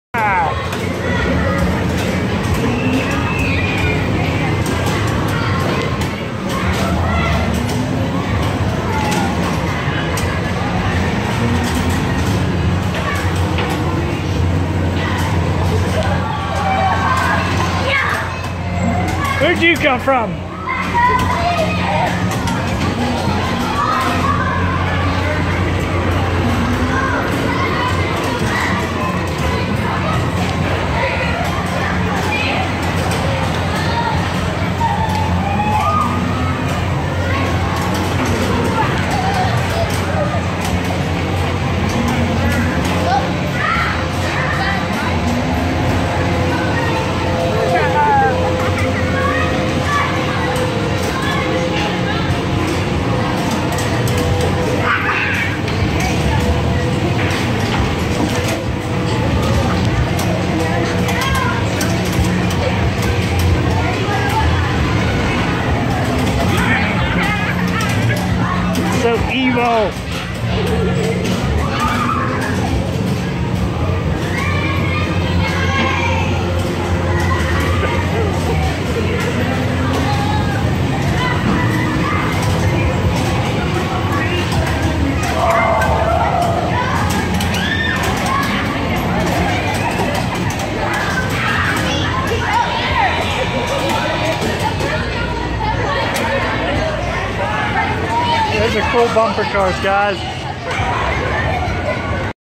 2017 Big Butler PA fair bumper cars
Really cool bumper cars at the 2017 big butler fair